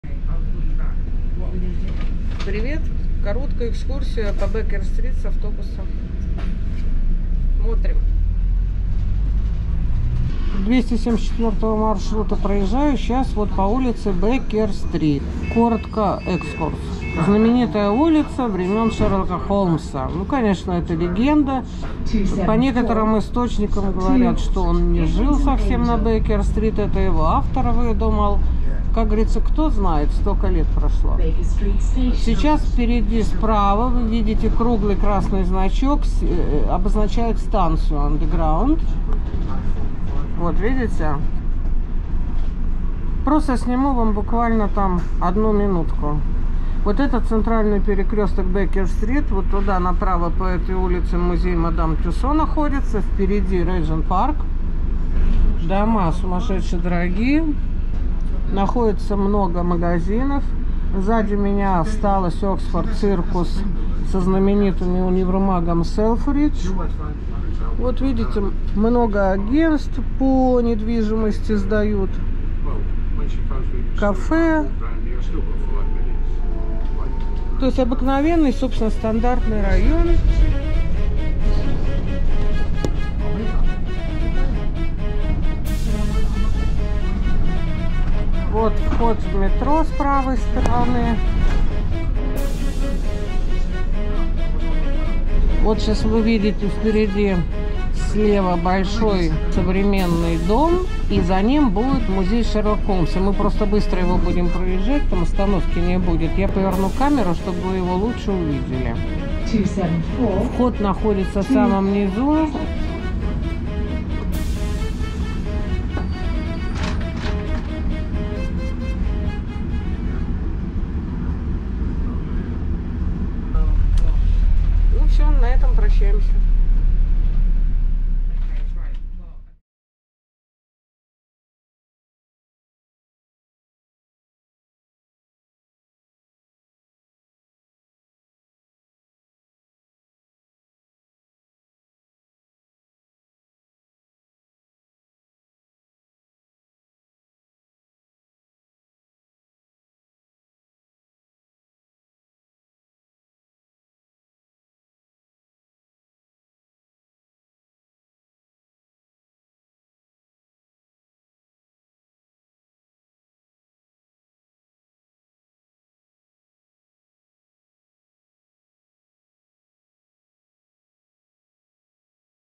бекер стрит 20 мая 10 утра
В 1990 году было принято решение открыть музей Шерлока Холмса в доме №239 потому, что возникло затяжное разбирательство за право владения культовым адресом между городским советом и Abbey National. После нескольких лет тяжбы банк прекратил своё существование. На здание торжественно установили табличку с прославленным книжным адресом, хотя и выбивающимся из общей нумерации.